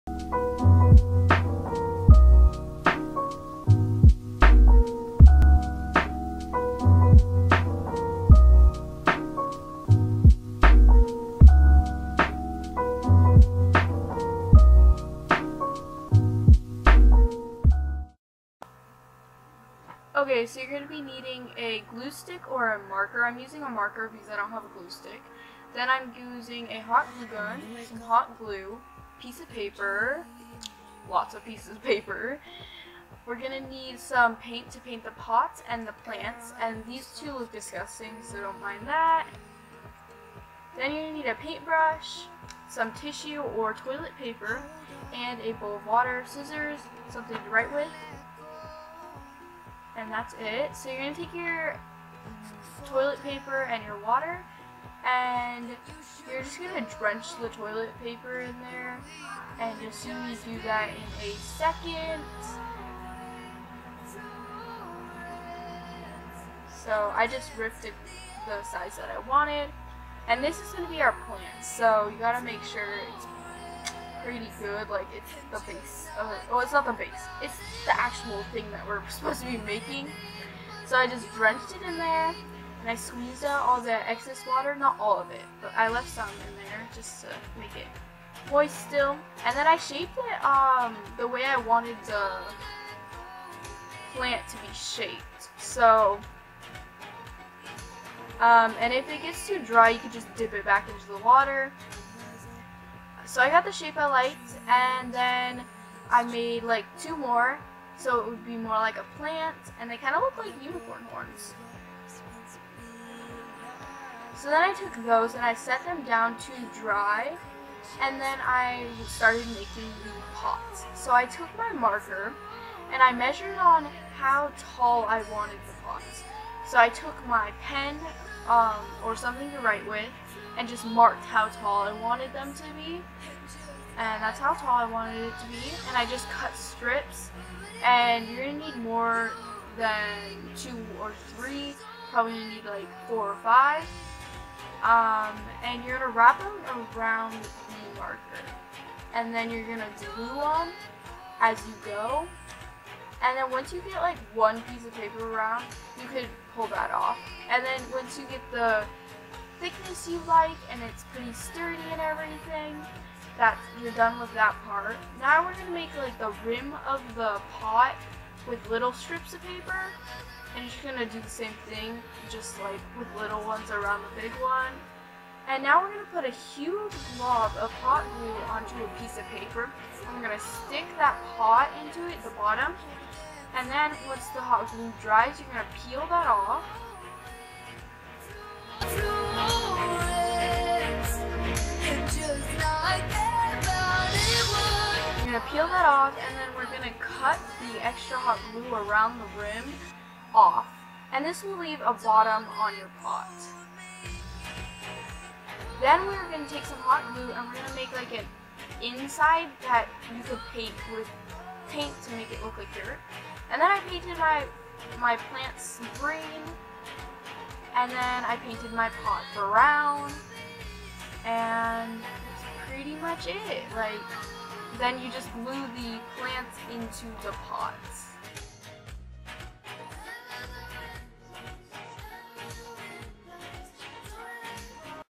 DIY lucianna study plants | Ag Unicorns
today I'm going to show you how to make these plants that Lucianna can study. if you look at the spacecraft (Lucianna's collection) you will notice that there are weird shaped plants. Also in there, the plants are different shapes and colors.